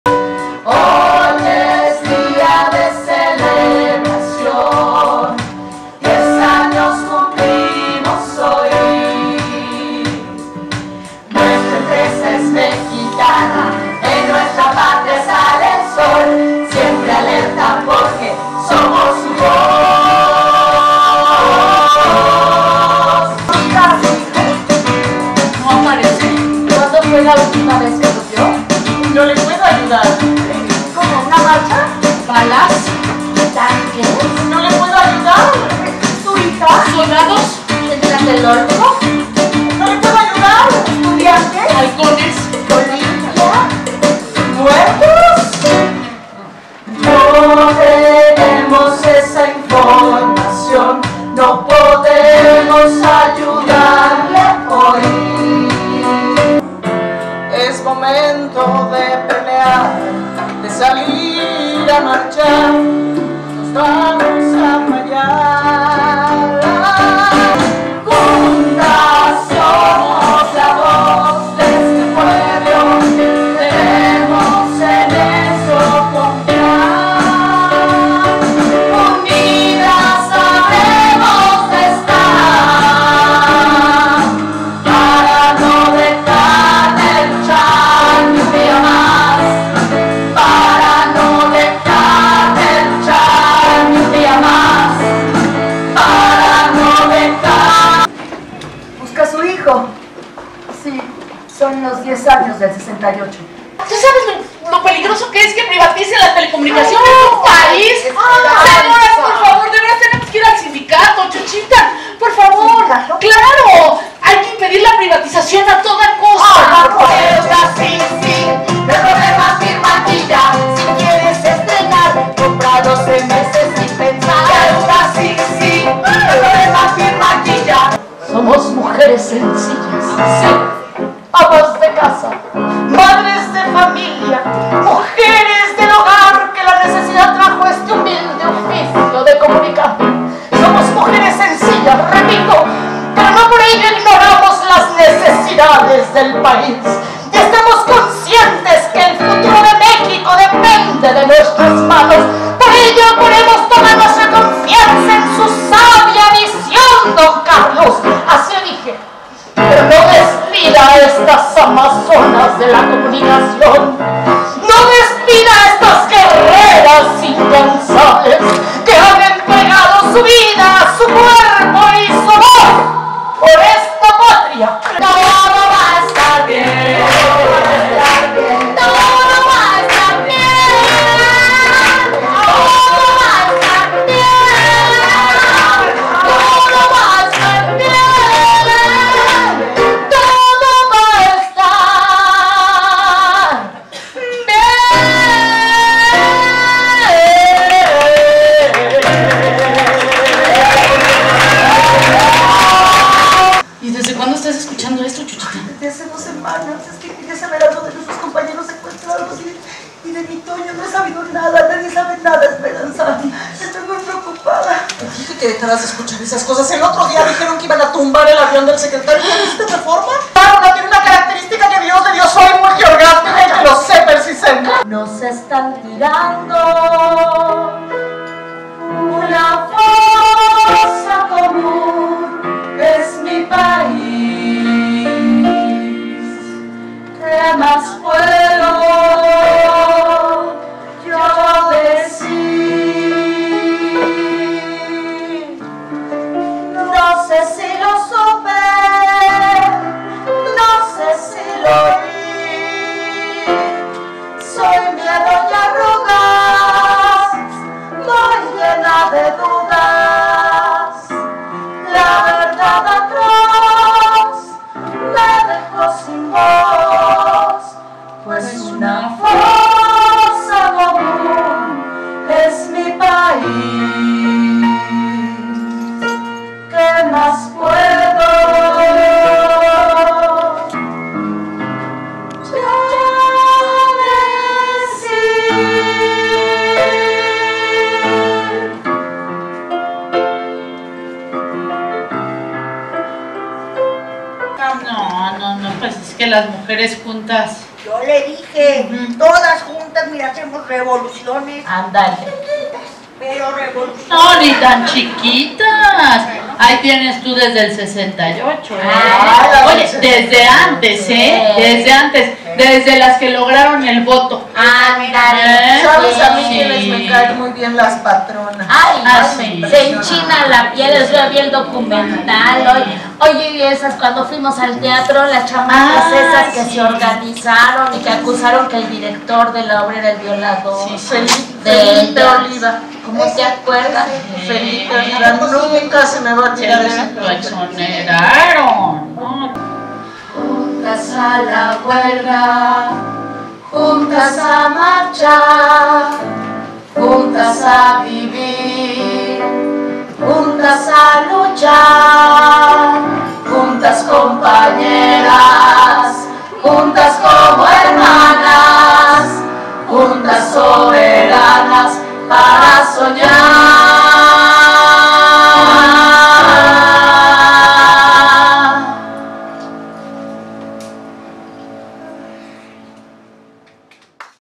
"A CHUCHITA SÍ LA BOLSEARON..."
"A CHUCHITA SÍ LA BOLSEARON, SÍ LA LLEVARON AL BAILE Y SÍ LE HICIERON DE CHIVO LOS TAMALES"

de LAS REINAS CHULAS

Resumen del espectáculo.
Homenaje al Movimiento del ´68.
México, 2018